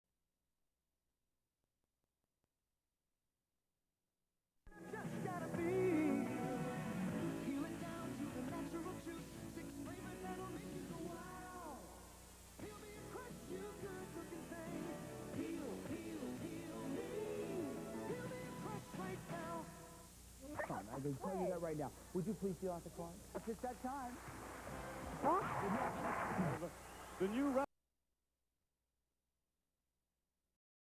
Crush Commercial 1987 ' Peel Me A Crush ' with Victoria Rowell
Crush Soft Drink Commercial 1987 ' Peel Me A Crush ' with Victoria Rowell (Drucilla Winters from Y&R)